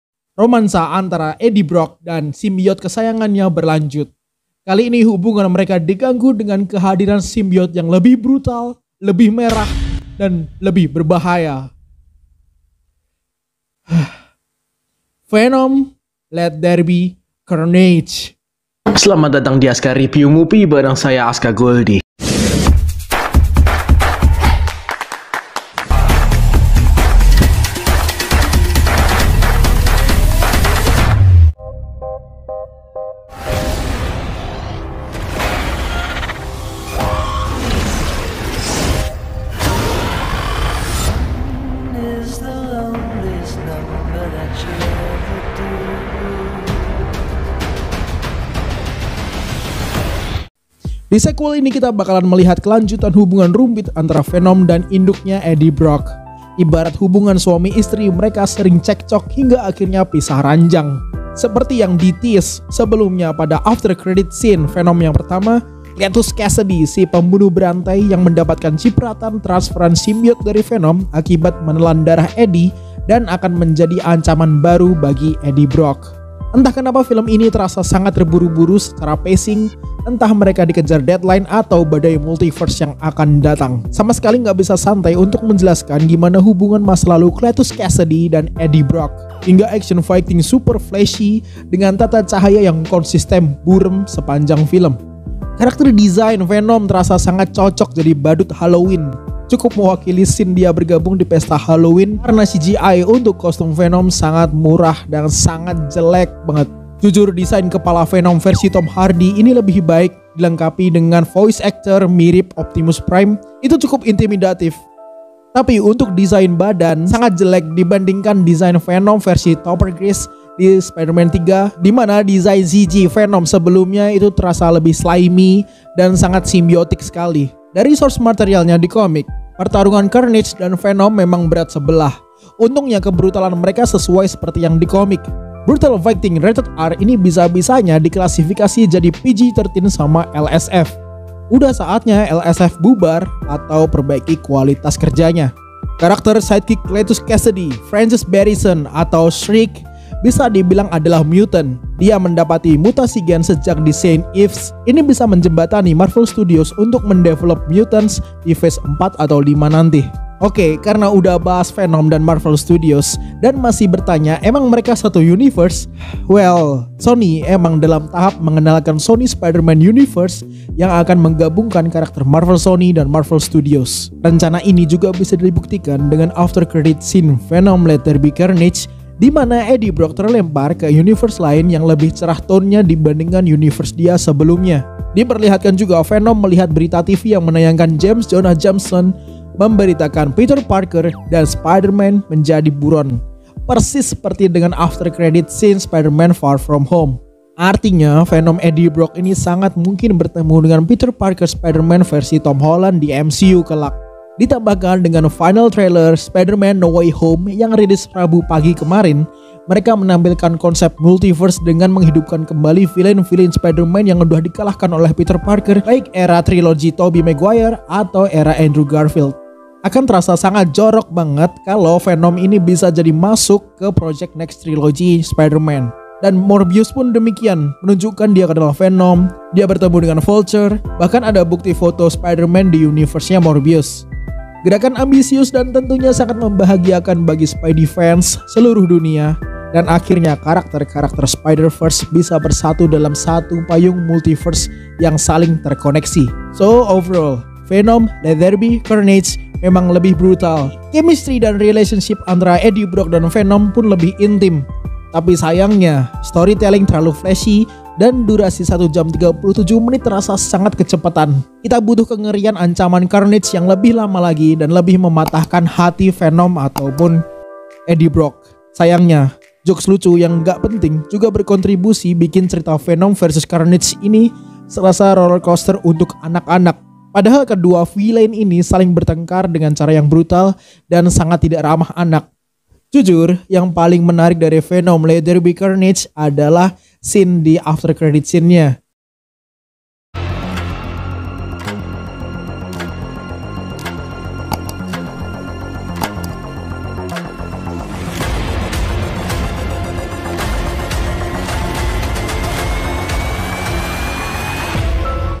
VENOM: LET THERE BE CARNAGE (2021) Review | Venom muncul setelah No Way Home !
Sebuah ulasan Film berbahasa Indonesia ala Azka Goldi di azkarepmu, Azka Repiuw Mupi!

Title:
Venom Let There Be Carnage
STARRING :
Tom Hardy, Woody Harrelson
DIRECTOR :
Andy Serkis

Terima kasih!